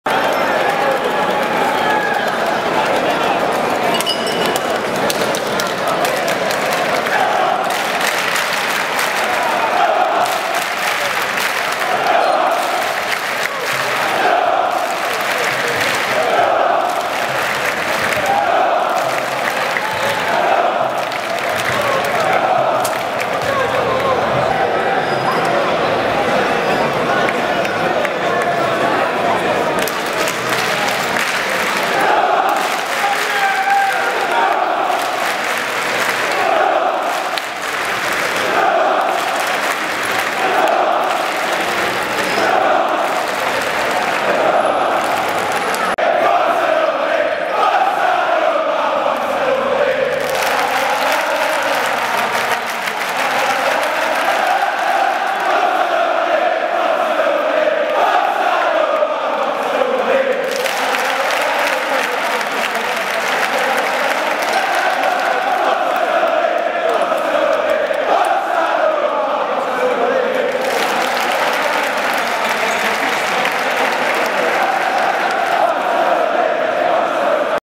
90 anni AS Roma, la festa al Pantheon: battimani e cori
I tifosi raggruppati al Pantheon prima di recarsi in Via degli Uffici del Vicario.